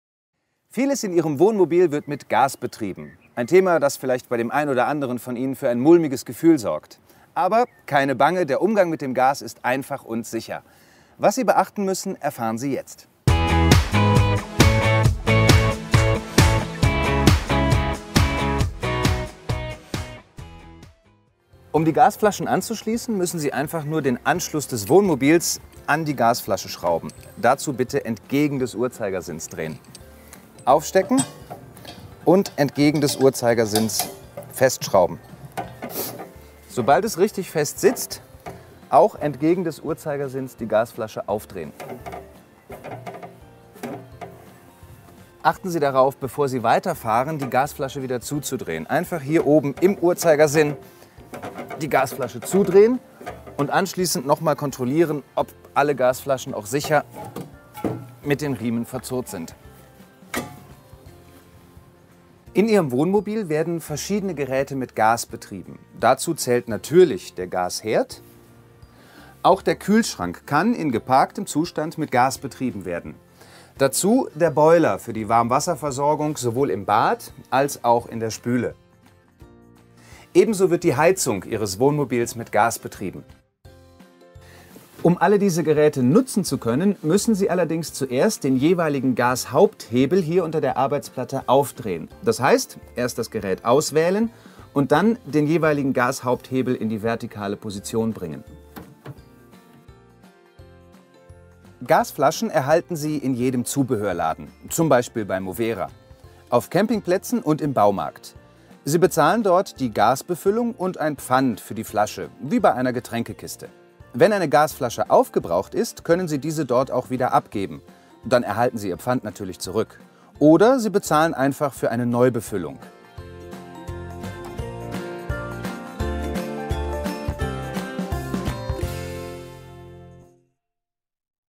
Carado-Ratgeberfilme für Reisemobile III. Gas und Gasflaschen
Carado Ratgeberfilme - Gas und Gasflaschen

In Ihrem Carado Reisemobil werden einige wichtige Dinge mit Gas betrieben.
Dazu zählt das Gaskochfeld, die Heizung, der Boiler zum Erhitzen des Wassers für Waschbecken und Dusche und optional der Kühlschrank im geparkten Zustand.
Der Umgang mit Gas ist sicher und einfach. Wir zeigen Ihnen wie Sie Gasflaschen korrekt am Wohnmobil  anschließen und verwenden und wo Sie neue und gebrauchte Gasflaschen kaufen bzw. befüllen lassen können.